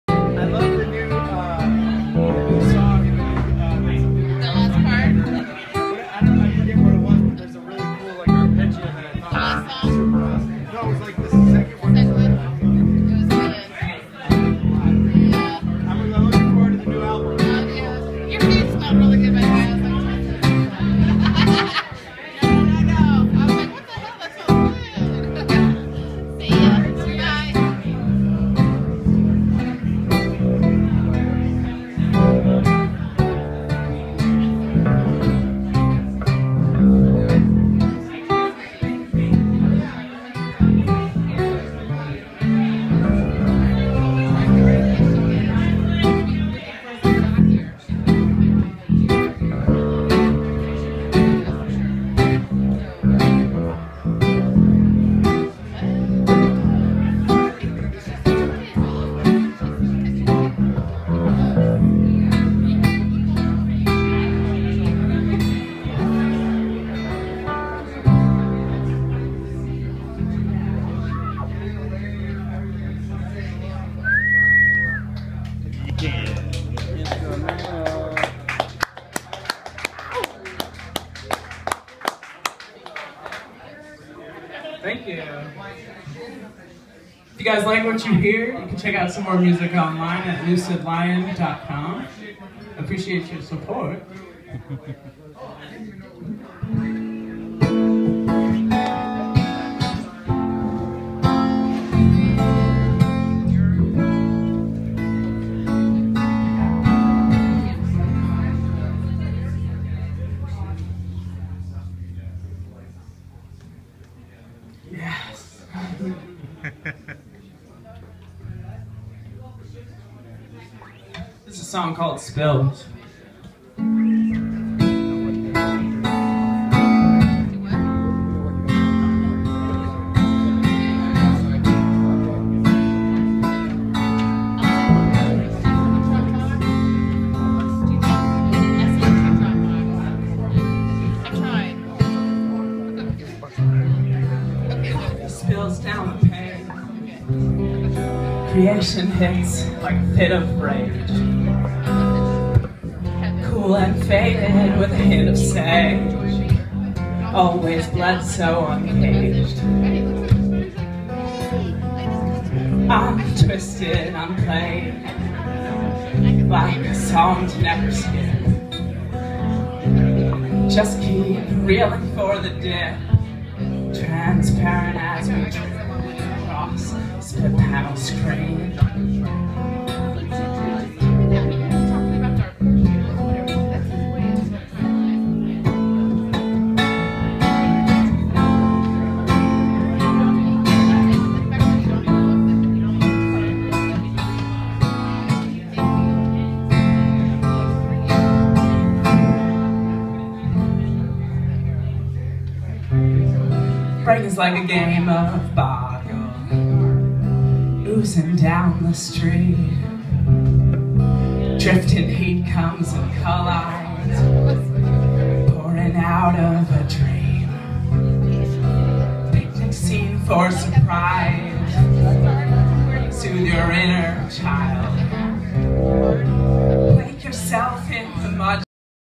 Lucid Lion ft. George Kazakos Live @ The Mint 12/04
Lucy Morningstar / Lucid Lion
Live at The Mint
Sunday 12/04

Video by Michael Brown -3